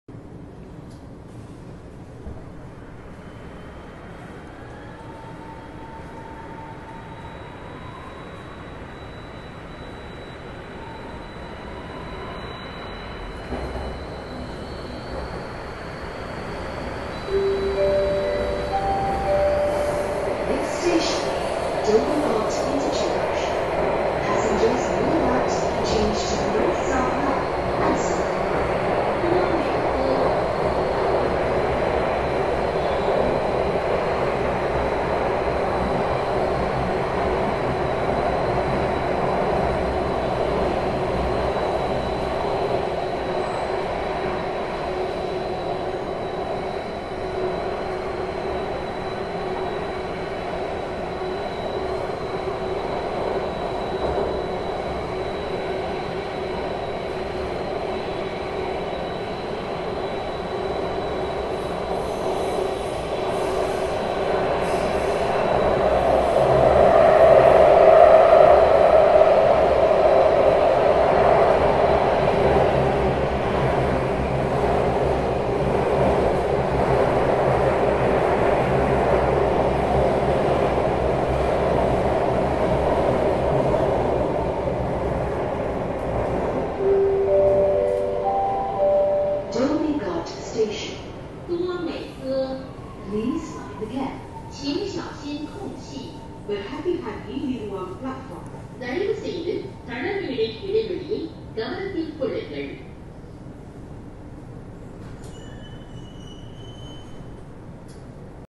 SBS Transit C751C 7063/7064 NE5 Clake Quay to NE6/NS24/CC1 Dhoby Ghaut (→ NE17 Punggol)(7)
Flimed on 3 June 2022, 7:13AM
Record by Kadin (owned by him)
Train and model: C751C And Alstom Metropolis
Set: 7063/7064
Train terminate: NE17 Punggol
Stretch: Clake Quay to Dhoby Ghaut
Route: Harbourfront to Punggol
A member of SG Trains